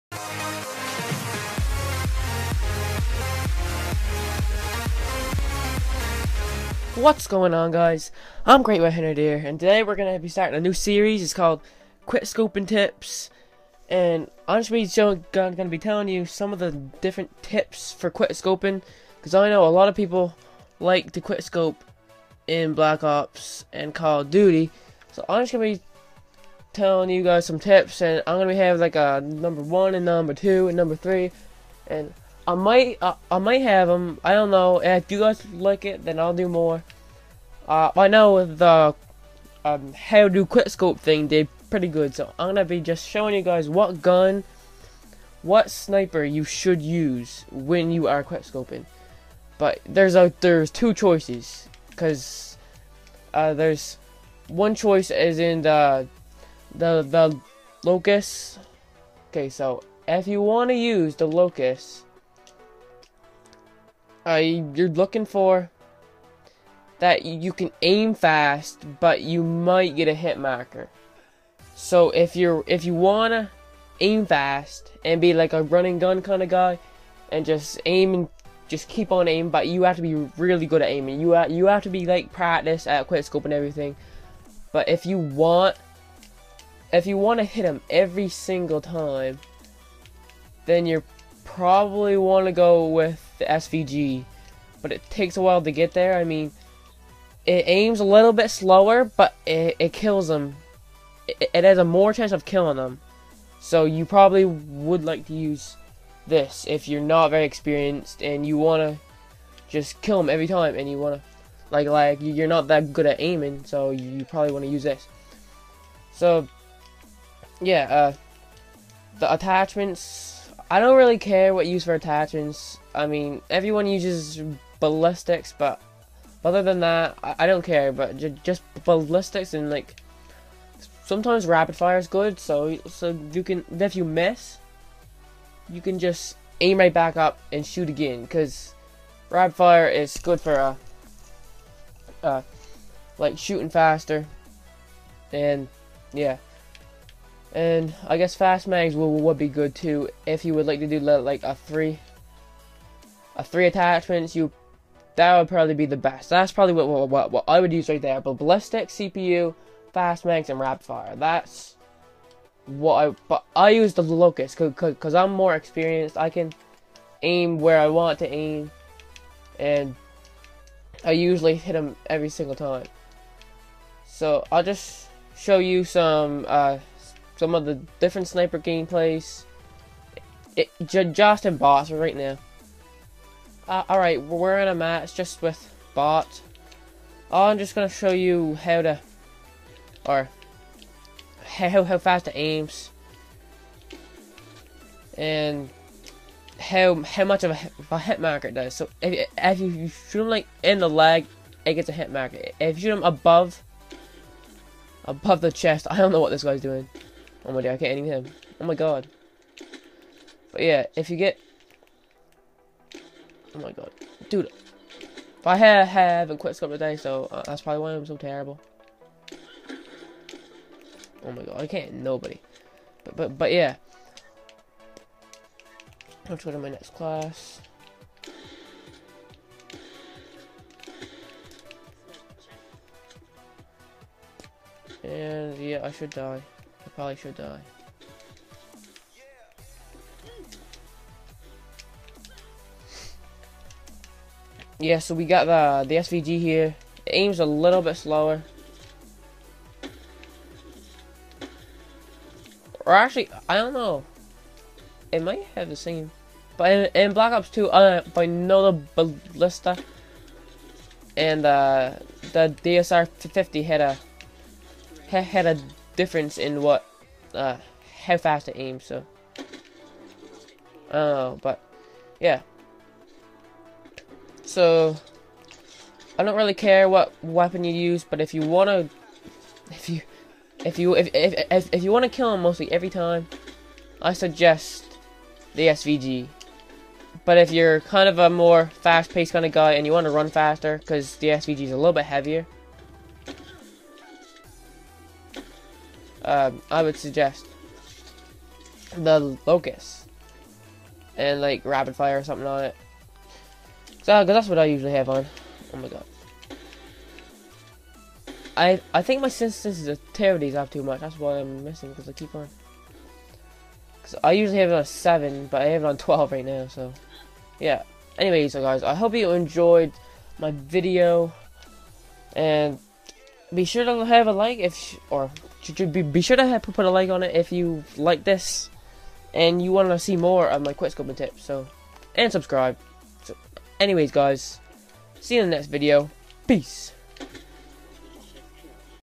QuickScoping Tips #1 - The Right Sniper!
Hey guys it's GreatWhiteHunter here and today I have a Quickscoping tips part 1 video for you guys! Peace!